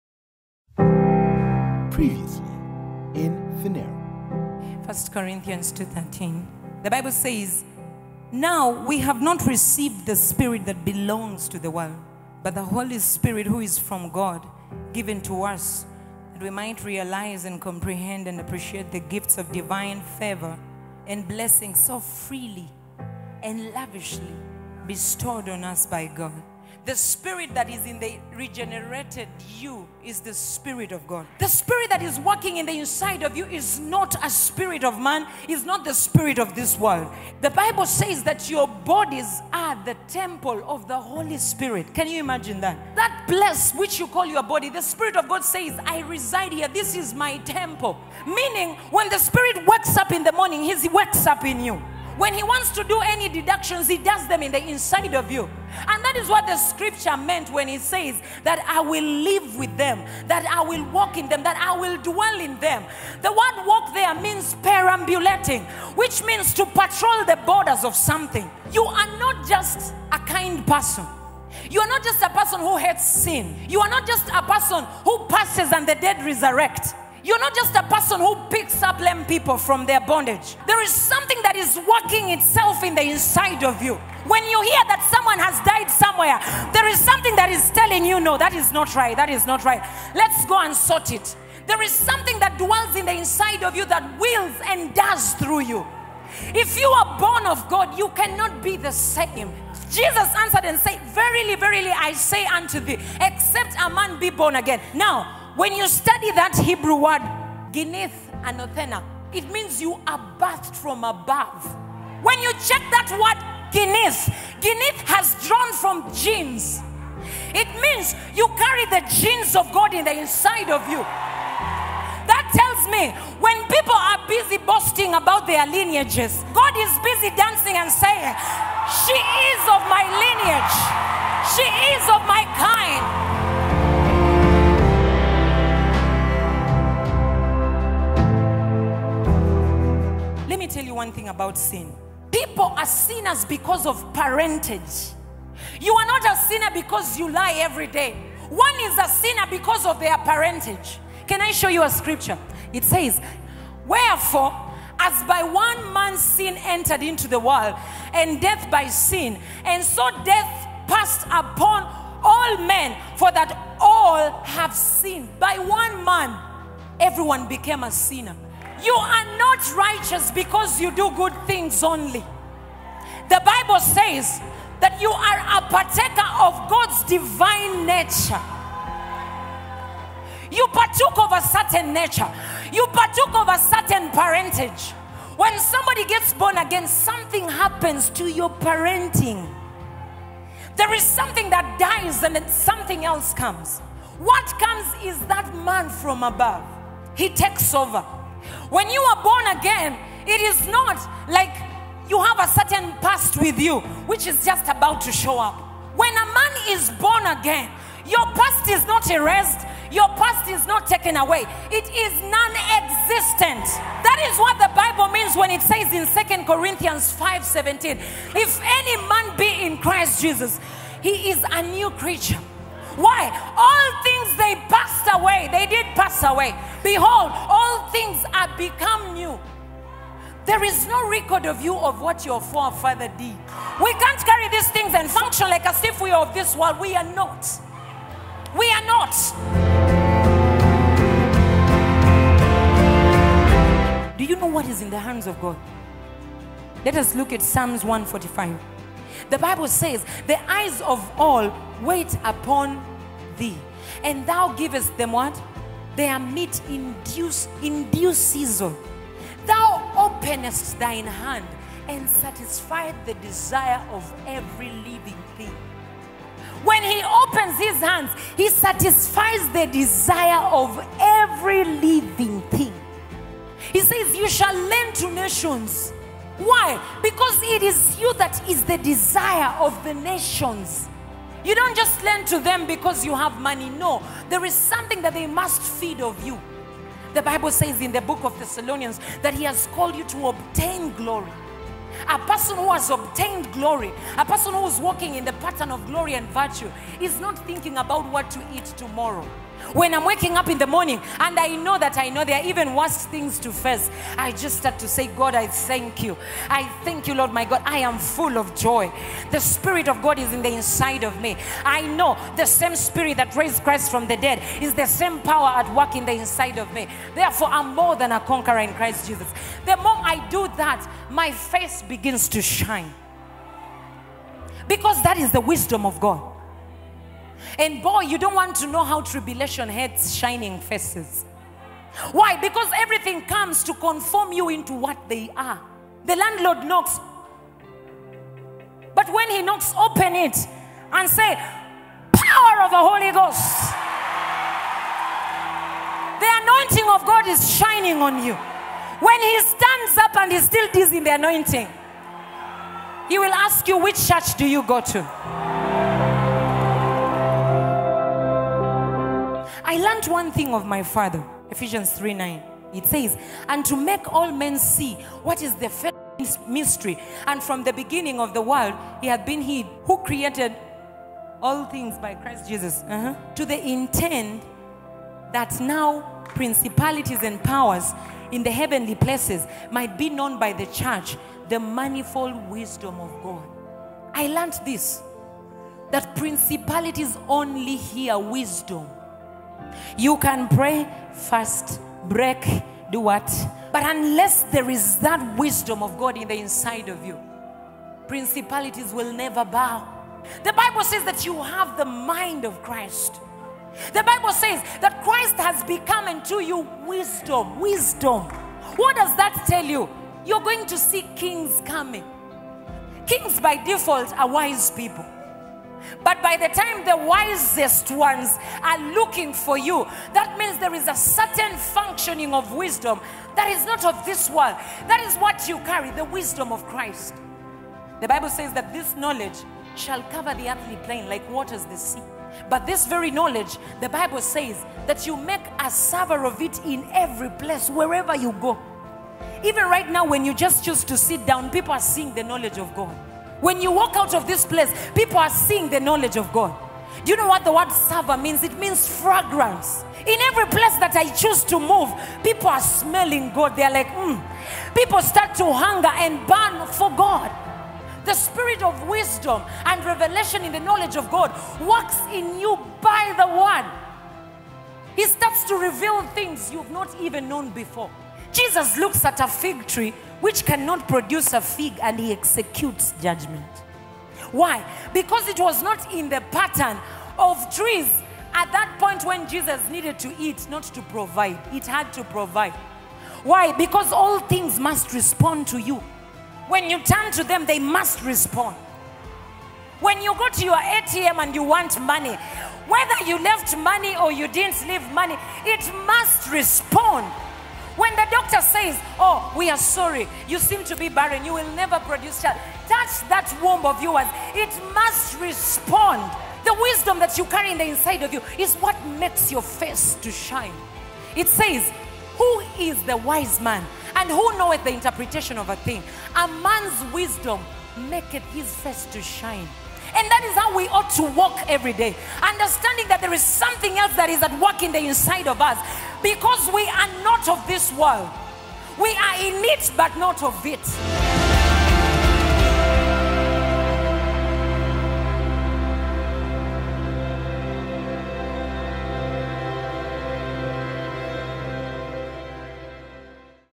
Authority In The Life | Sermon Preview | Pastor Modesta Sweeney
Authority In The Life | Phaneroo Service 140 Sermon Preview by Pastor Modesta Sweeney

The life from above is where the life of God is, there is authority to execute judgement.